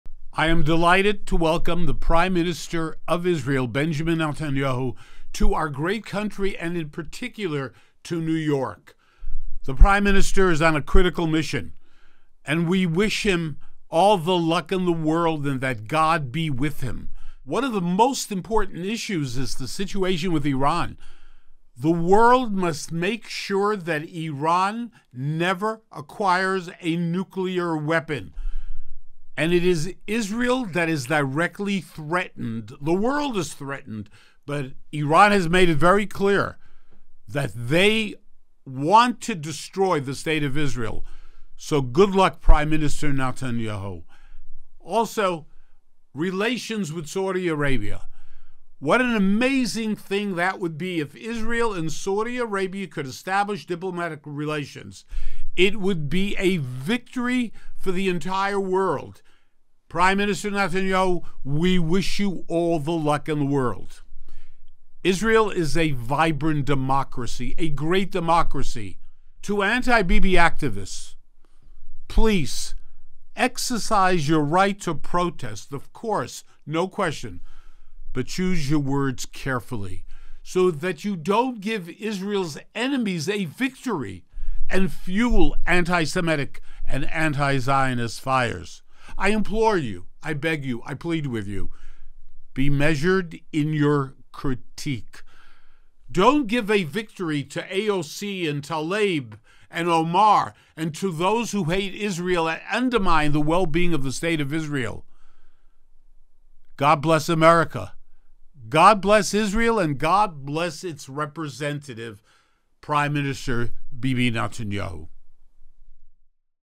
Dov Hikind Welcomes PM Netanyahu to NYC and Warns Anti-Bibi Activists of Fueling Anti-Zionism
Amidst all the noise surrounding Benjamin Netanyahu's trip to the United States, and New York City in particular, Dov Hikind welcomes the Israeli prime minister, wishing him well in his mission to protect Israel's citizens and interests, and warns the well-funded organized anti-Bibi effort comprised of Israeli expats to be careful in their public critique and spectacles because the whole world is watching, particularly Israel's enemies for whom anything negative about Israel is welcome fuel for their antisemitic fires.